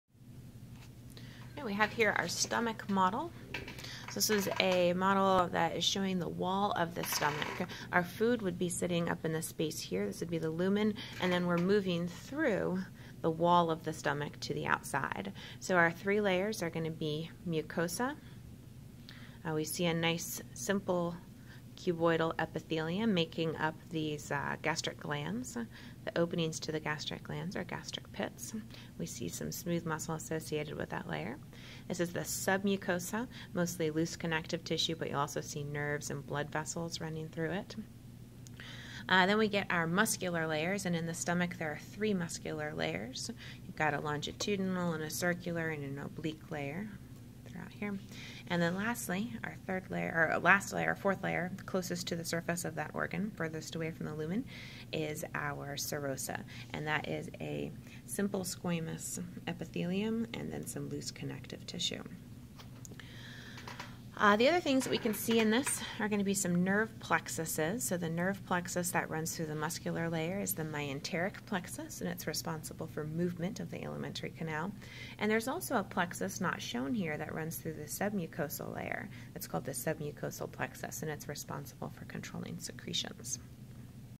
Stomach wall model
Note the error - gastric glands are composed of simple COLUMNAR epithelium and not simple cuboidal.